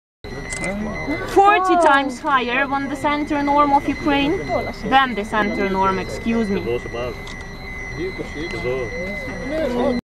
Geiger counter reading: Kopachi, near Chernobyl
Geiger counter reading in the village of Kopachi, in the 10-kilometer Exclusion Zone near Chernobyl.